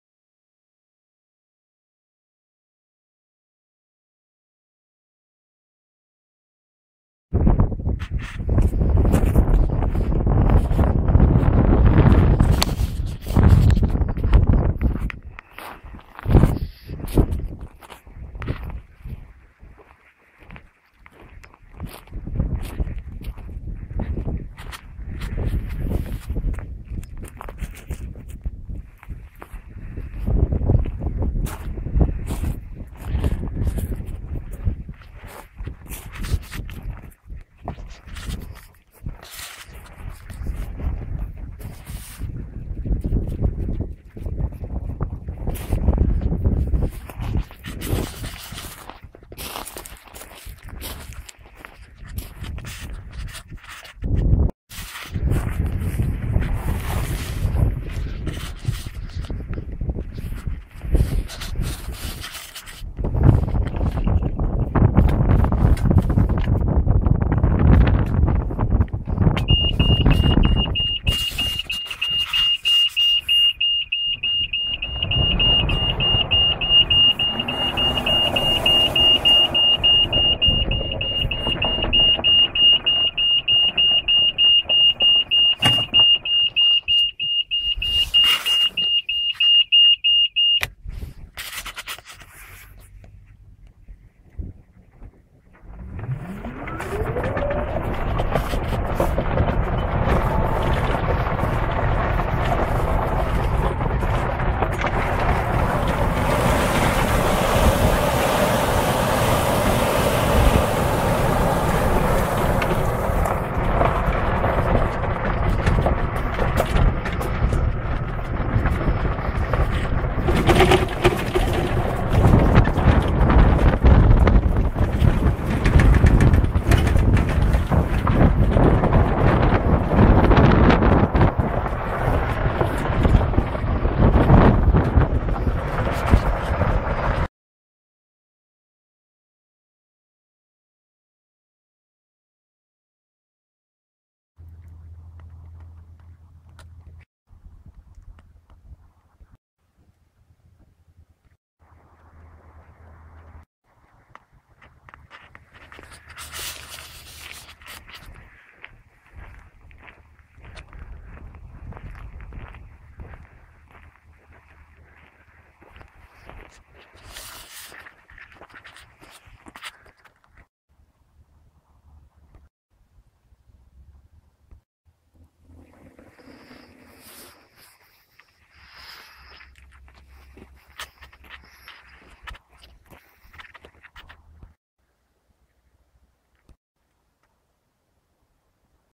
ᴴᴰ driving a Malex golf cart
This cart features the 48 Volt Sepex Curtis 1266 controller,Trojan T105 batteries, 4kW ADC motor and can be optionally supplied with Water Misers or AGM maintenance-free batteries.